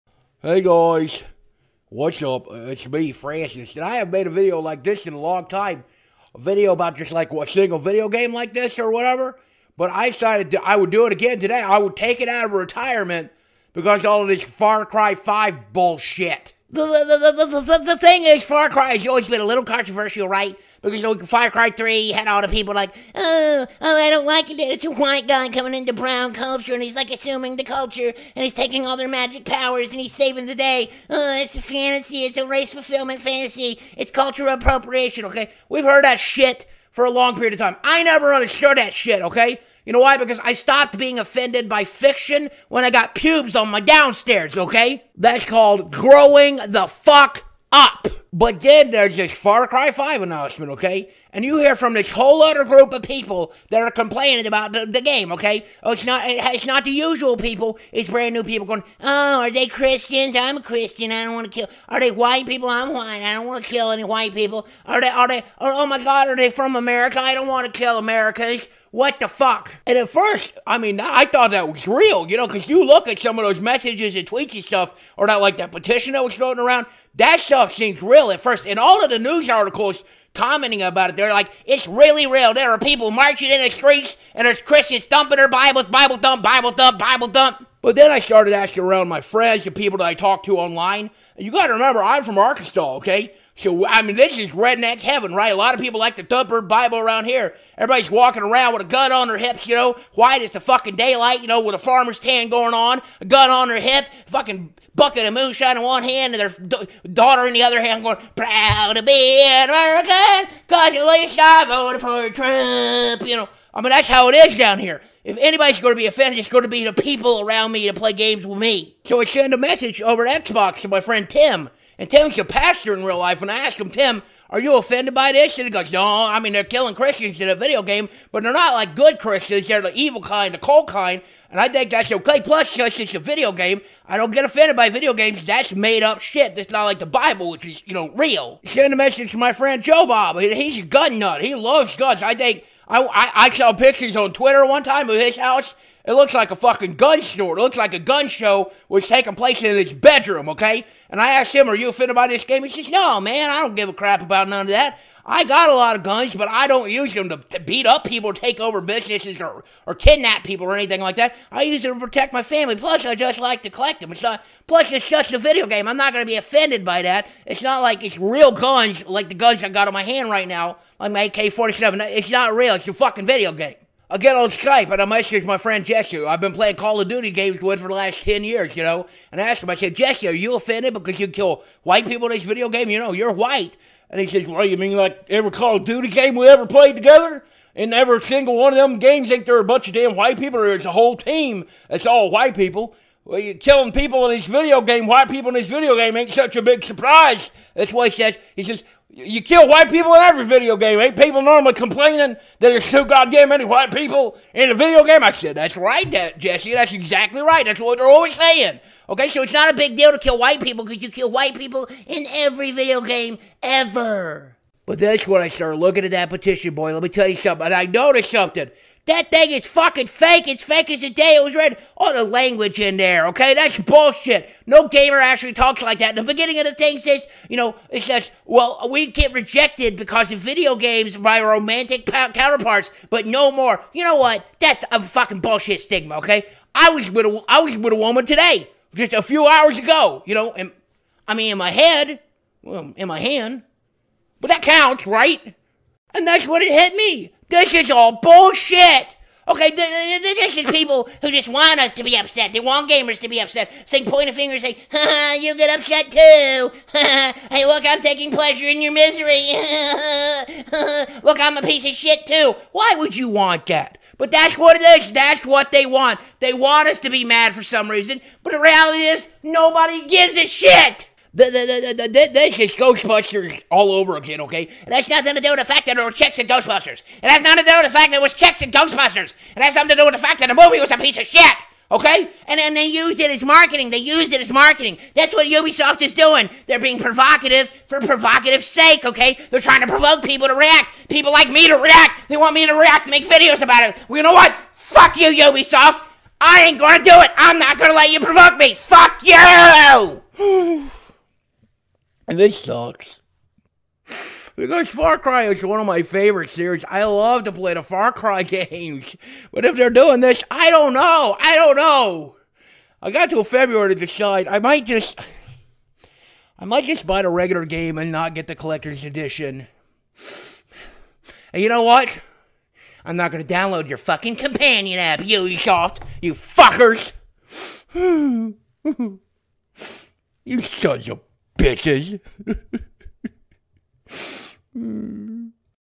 FRANCIS HATES FAR CRY 5! FRANCIS RAGE!!!
Francis has finally gotten clued in on the far cry 5 contraversy and how everyone seems to be upset when no one actually seems to be actually upset.